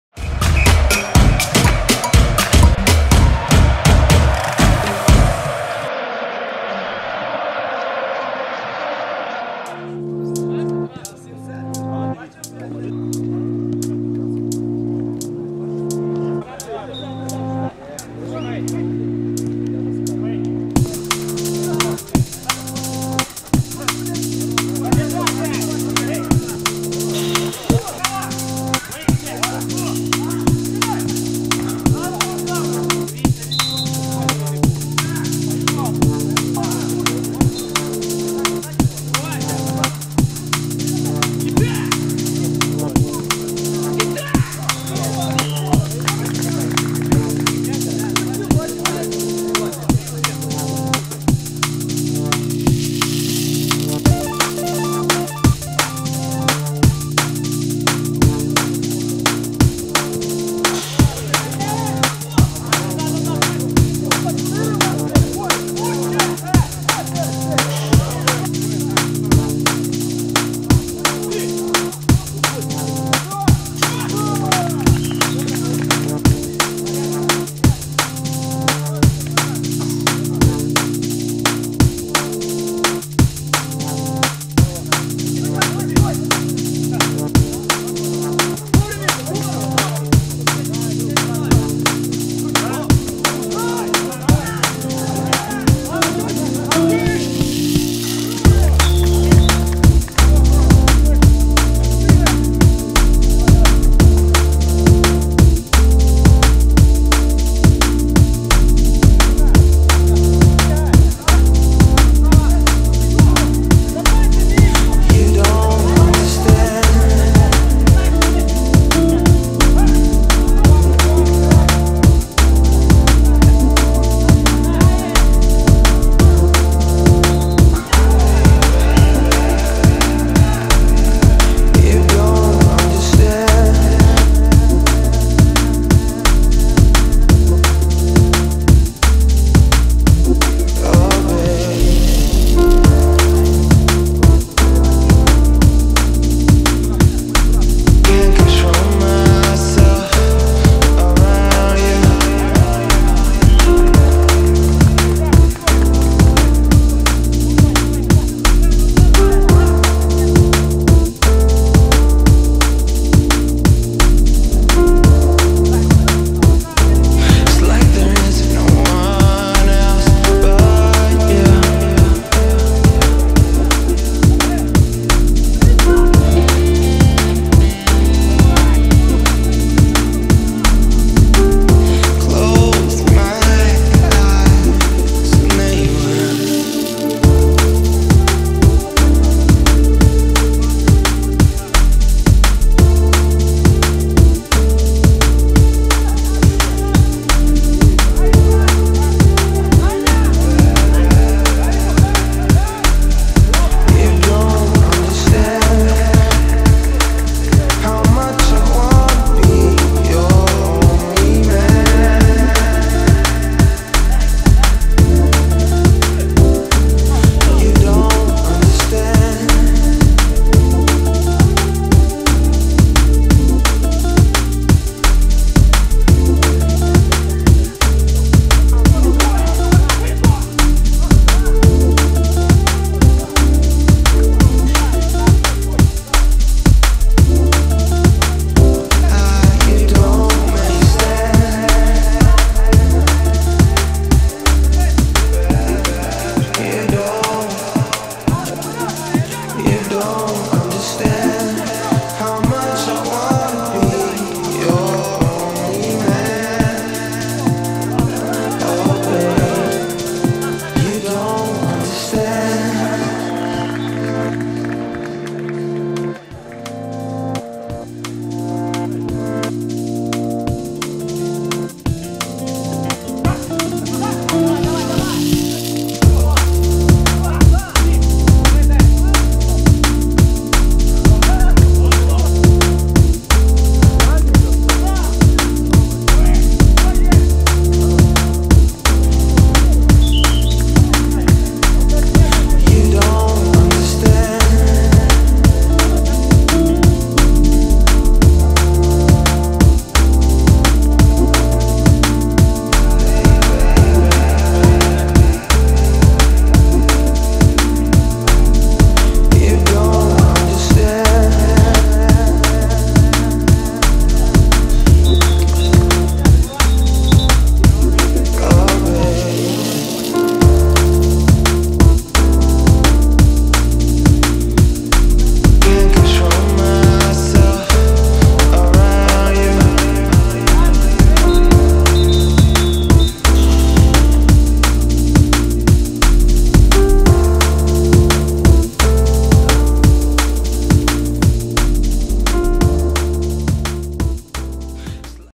Спільнота - Інкогніто [Огляд матчу] (Перша Ліга. 8 тур)
Перша Ліга міні-футболу Львова (літо 2015)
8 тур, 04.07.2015
Спільнота - Інкогніто 4:6